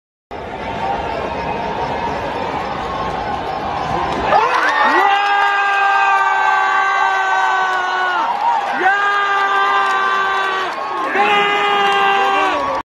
Dhoni Hits Massive 6 against KKR IPL 2022 Match-1
IPL 2022 Match 1 Highlights Chennai Super Kings vs Kolkata Knight Riders | Ipl 2022 match 1 highlights csk vs kkr

Images/Visuals Credit: BCCI